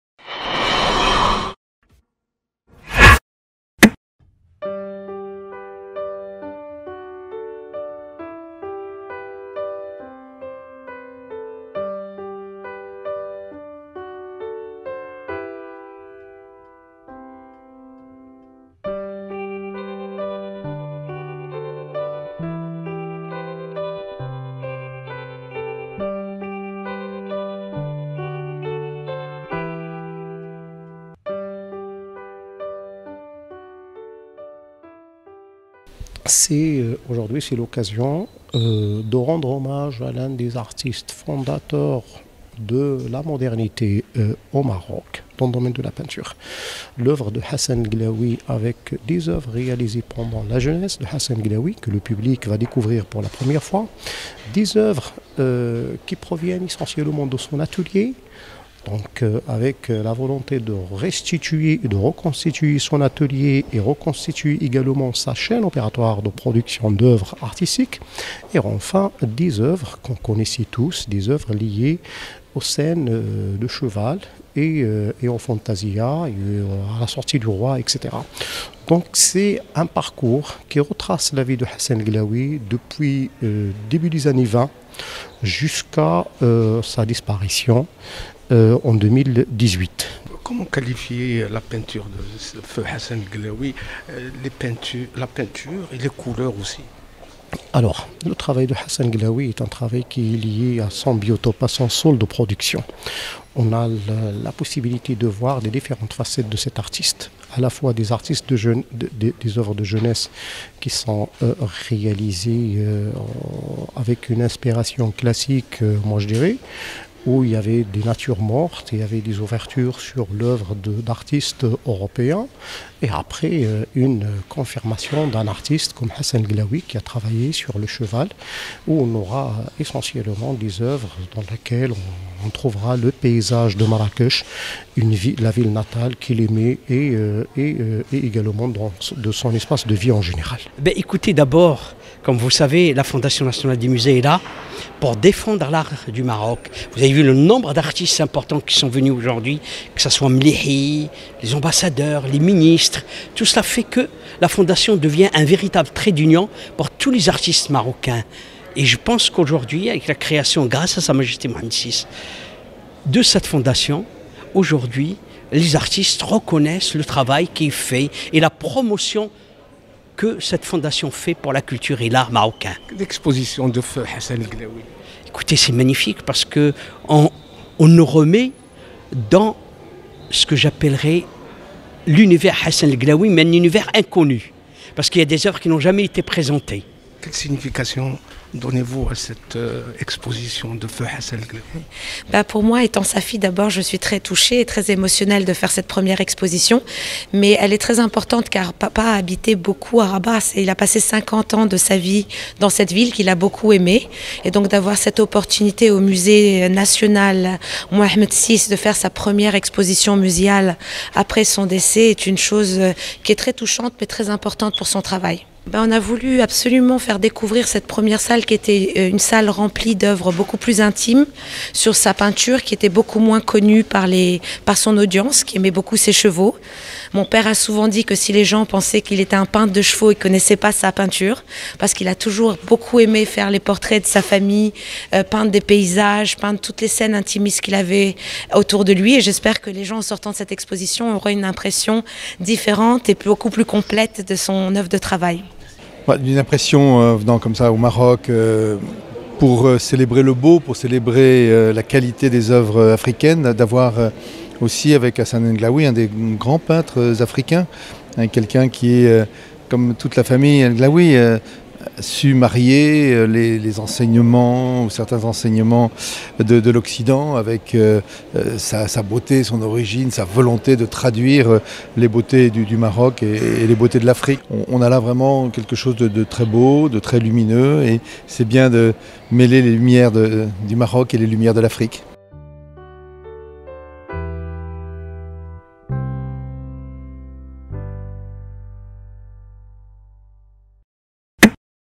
Hassan El Glaoui tribute to an indisputable master of figurative painting
The National Museums Foundation (FNM) presents, on the sidelines of the exhibition "Lights of Africa", the precious exhibition of Hassan El Glaoui. After paying homage to the great Moroccan masters, the FNM wishes to highlight the founding artistic trends of the second half of the 20th century. The Mohammed VI Museum welcomes, from April 2 to August 31, an indisputable master of figurative painting, since it was one of the first in Morocco. This is the late Hassan El Glaoui, in the theme "The Salt of my Earth".